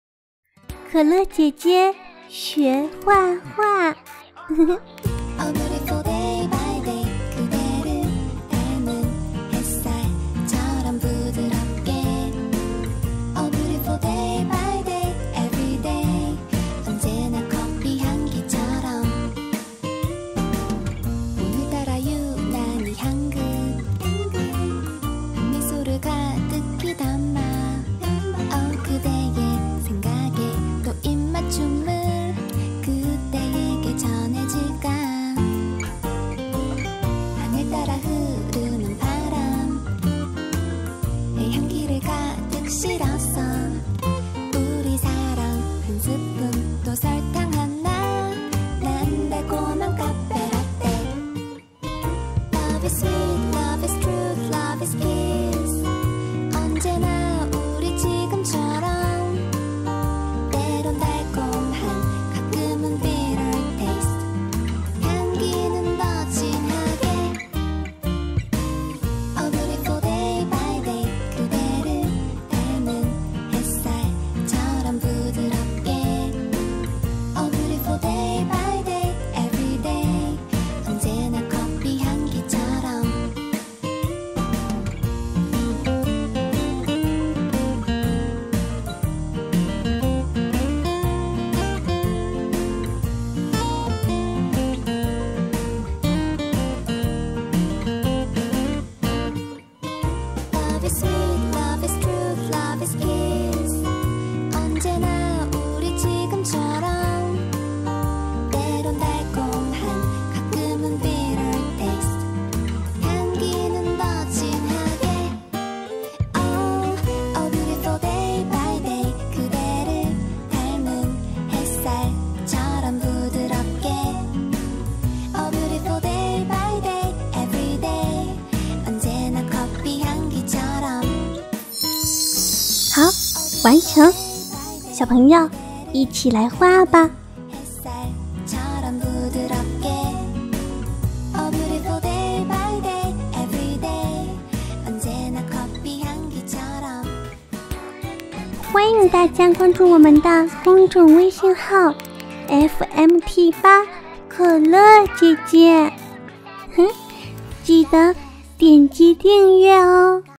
【可乐姐姐学画画】两只轻松熊
今天可乐姐姐要学画的是两只轻松熊，来吧我们大家一起动手画吧！ 我的公众微信是：fmt8kelejiejie，欢迎宝贝们关注！每晚都有精彩睡前故事伴随宝宝！